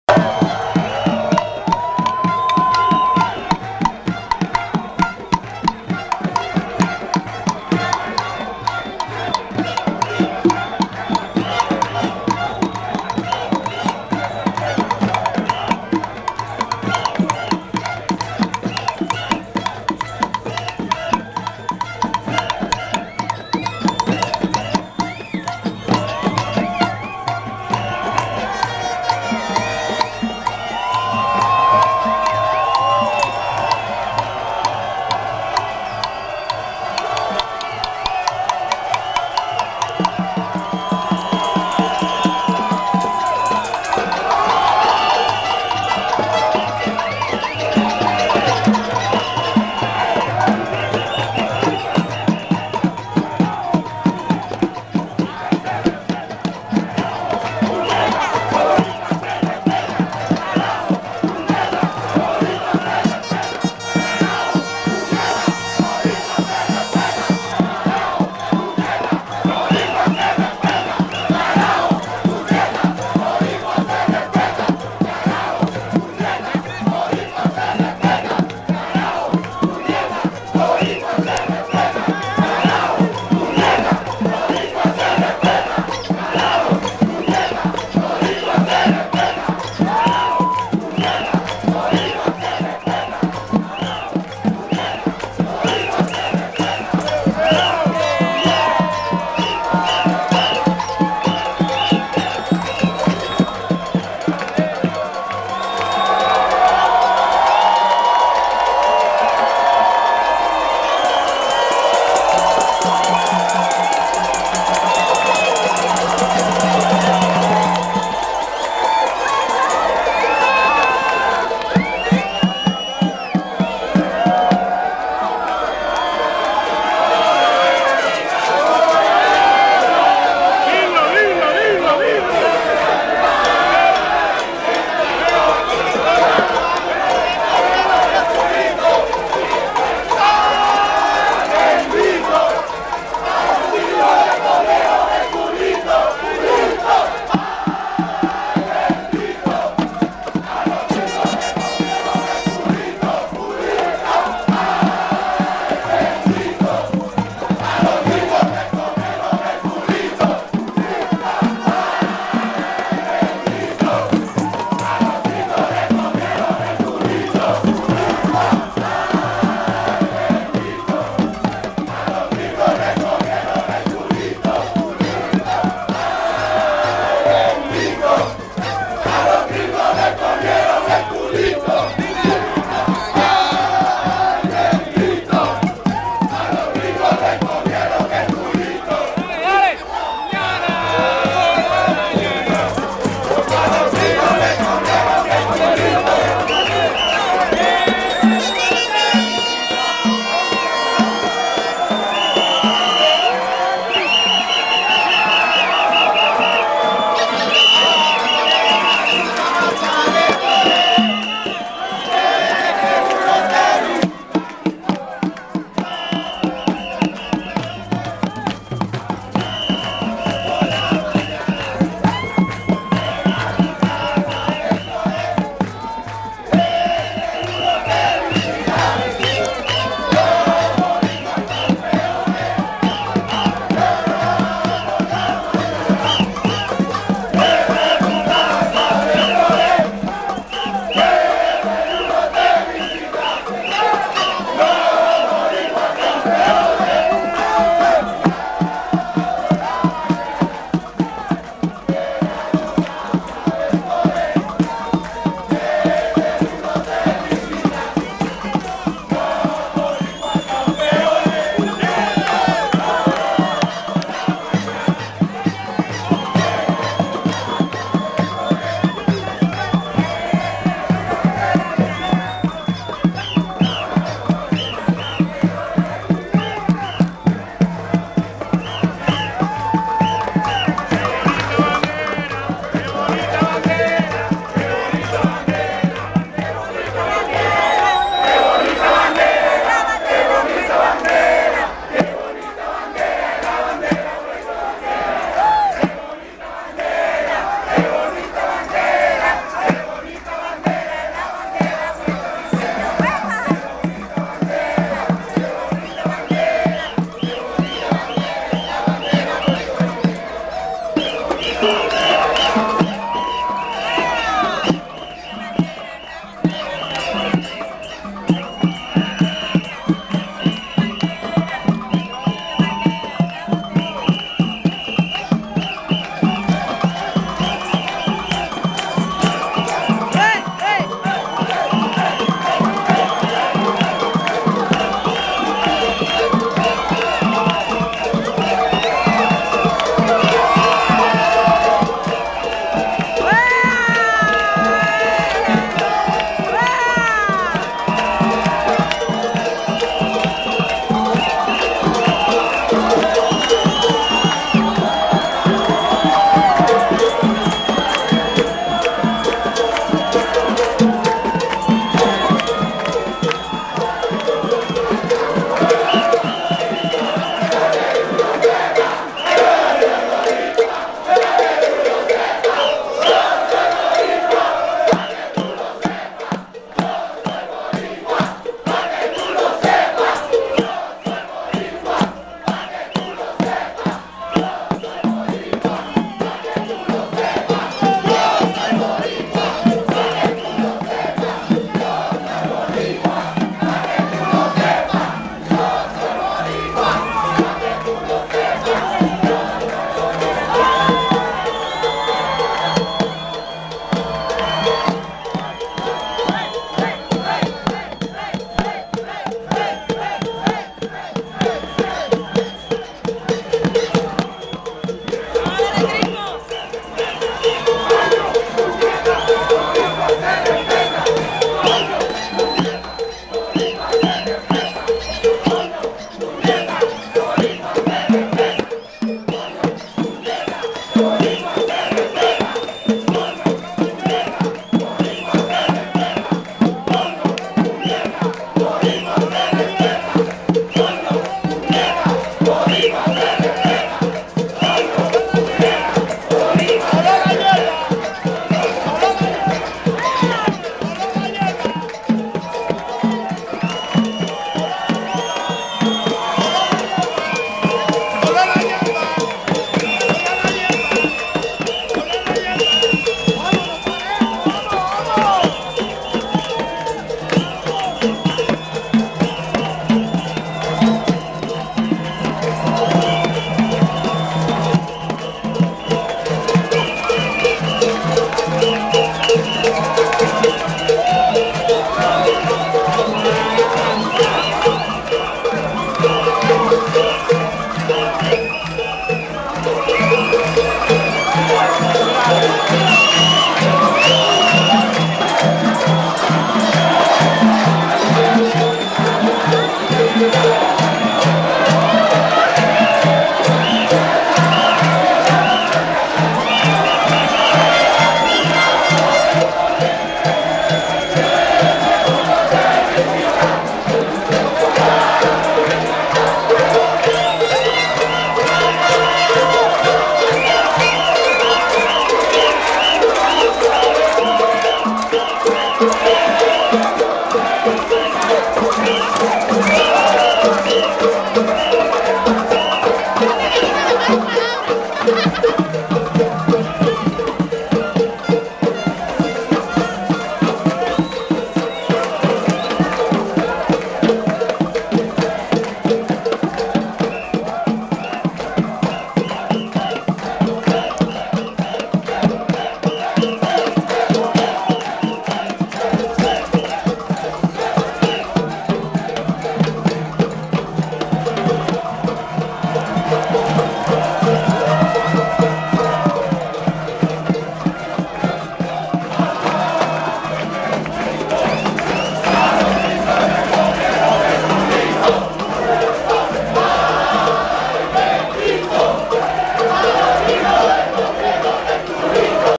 Puerto Rican Fans Celebrating Victory vs USA in the 2013 WBC @ Marlins Park (Pt 1)
with Joe Collado and Jackie Collado